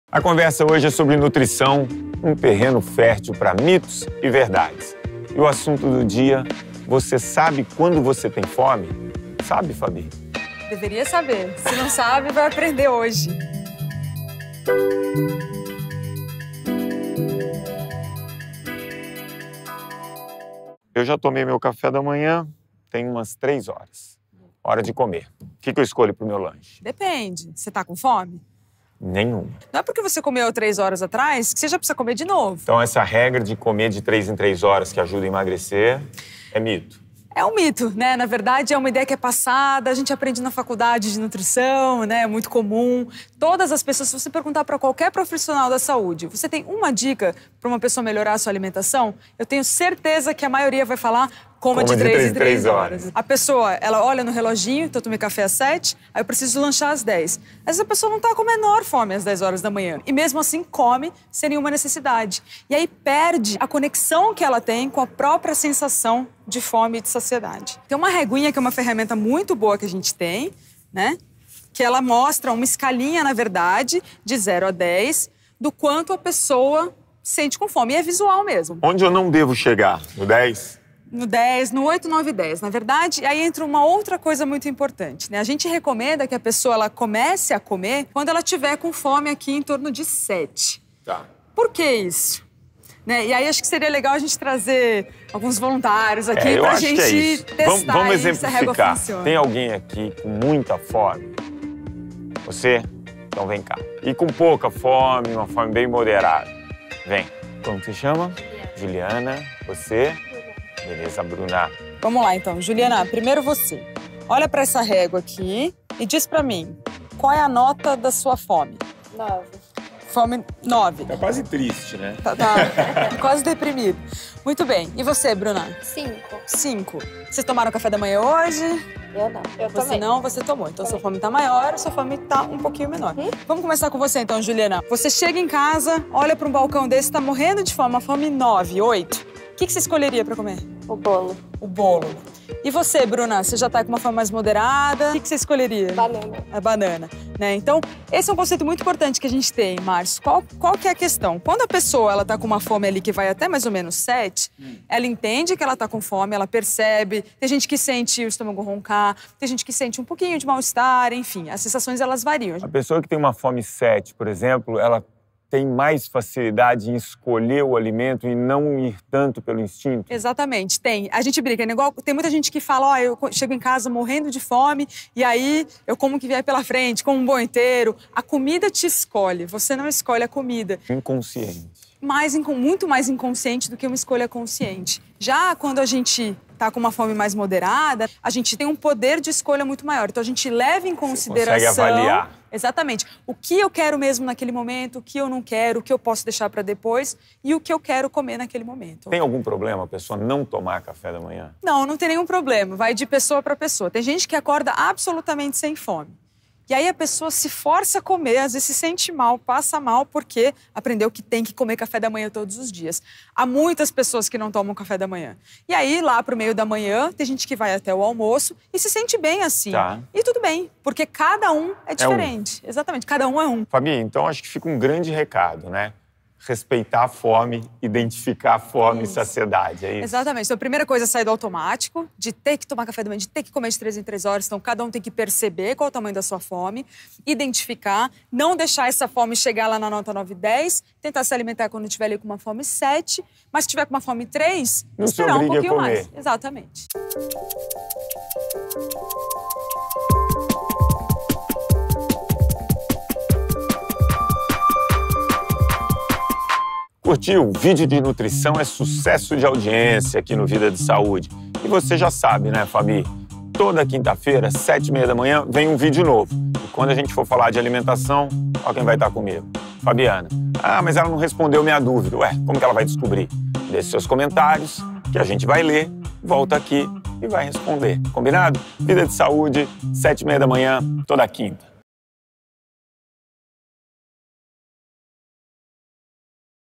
O QUE E QUANDO DEVO COMER?
Marcio Atalla e a nutricionista Fabiana Benatti dão dicas do que comer pra ter uma alimentação saudável!
Canal BemStar - vídeos novos segundas e quintas às 07h30.